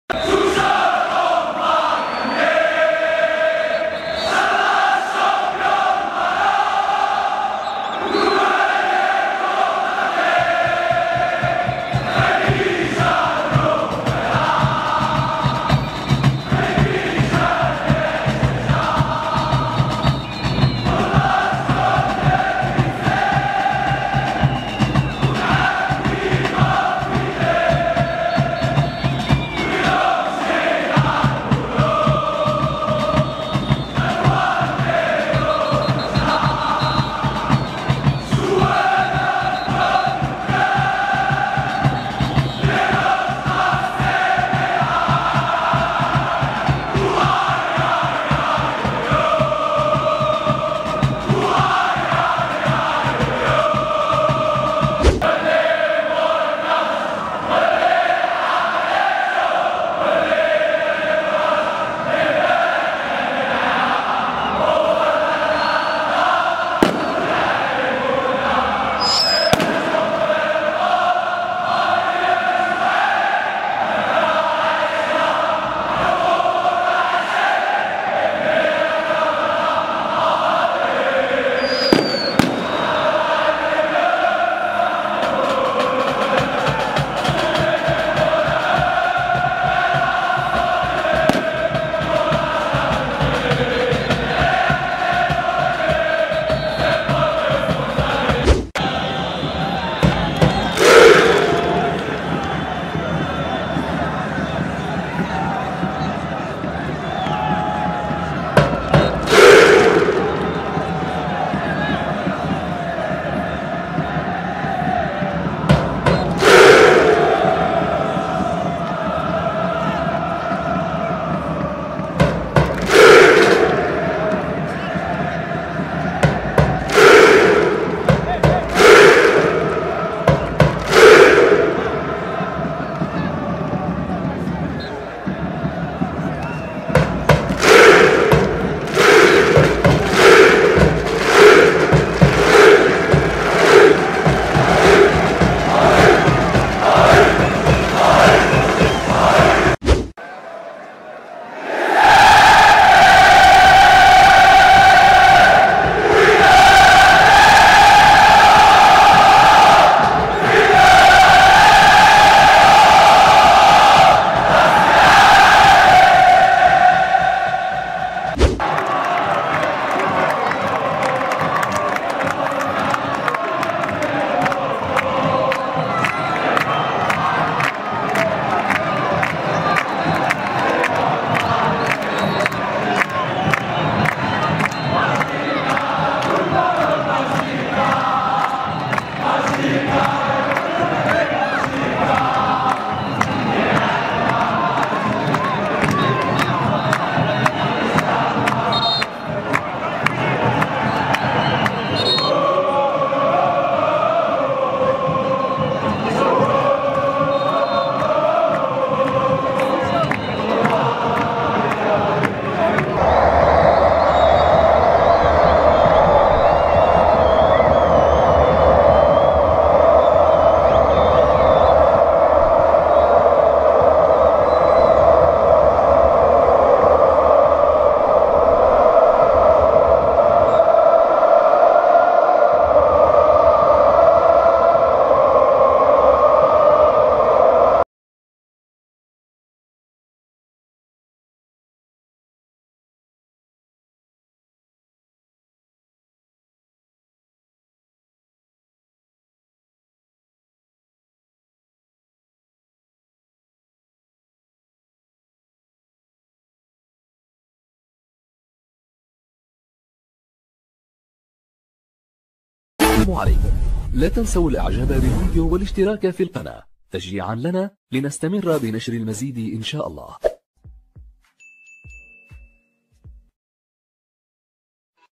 -عاجل مهارات وأهذاف اللاعب النرويجي المغربي الموجود في مركب بن جلون+مفاوضات جديدة مع أيوب الكحل
-النرويجي المغربي عمر الغوتي يتدرب رفقة الوداد الرياضي في بنجلون
-مفاوضات جديدة مع أيوب الكحل